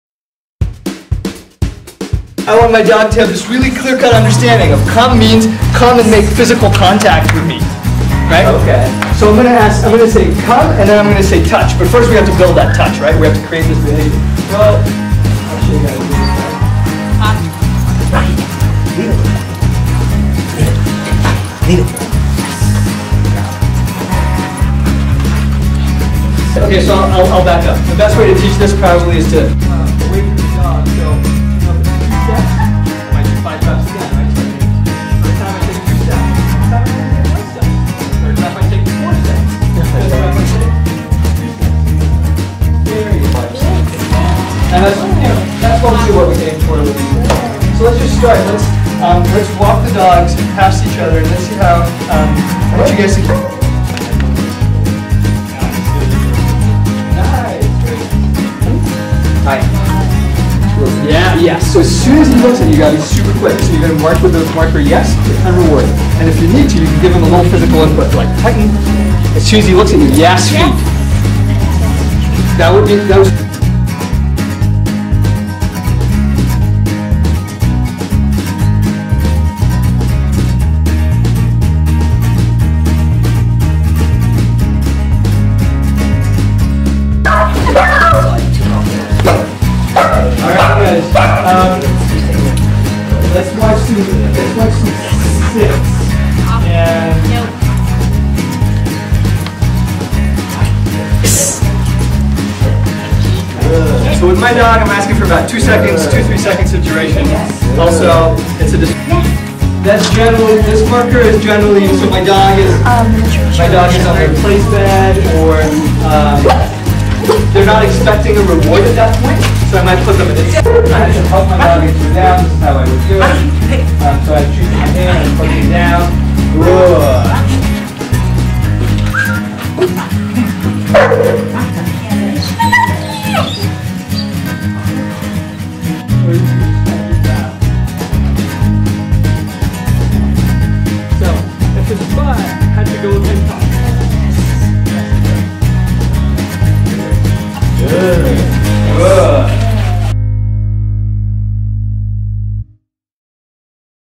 Dog House Phd. Dog Training Class Lecture
Filmed a DOG House Phd Lecture Class video based in Los Alamos, New Mexico. The video is cut to 2:34 seconds but was around 7 mins to just 14 mins so that is a lot of run time on a small class video. But I wanted a whole lot of footage to choose from and for the owner Mrs. Ruth to choose from two. I'll be posting two videos on Dog House Phd. so keep your eyes peeled for the next video installment.